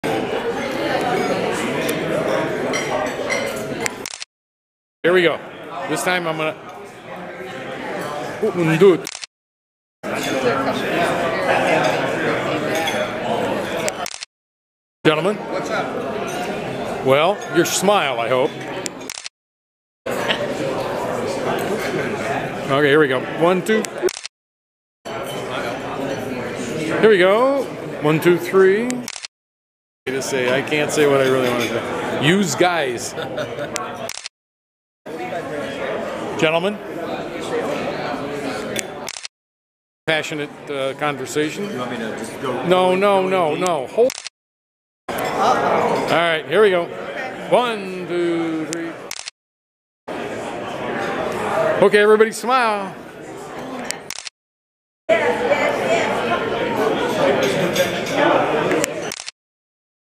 Toledo Rotary 08092021 Snippets
A sampling of Members and guests attending Toledo Rotary on Aug 9 Founder's Dining Room at the Toledo Club.